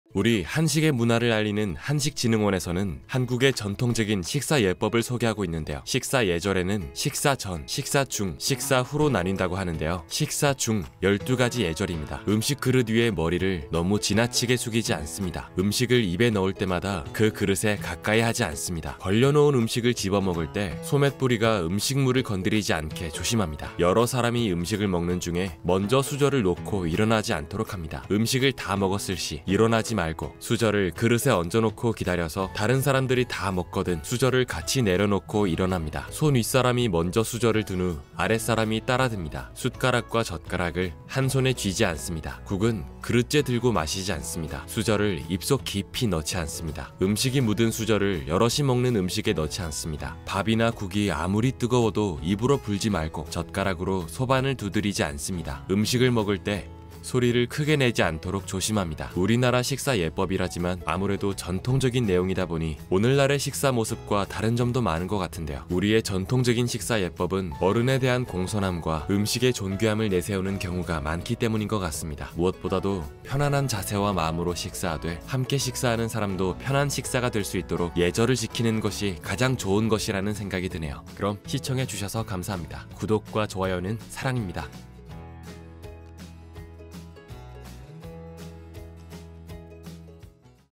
식사중 지켜야 할 12가지 예법! 얼마나 지키고 계신가요?
우리 한식의 문화를 알리는 한식진흥원에서는
한국의 전통적인 식사 예법을 소개하고 있는데요.
식사 예절에는 식사 전, 식사 중, 식사 후로 나뉜다고 하는데요.
‘식사 중’ 12가지 예절입니다.

[참고자료]
한식진흥원│식사중 지켜야 할 12가지 예법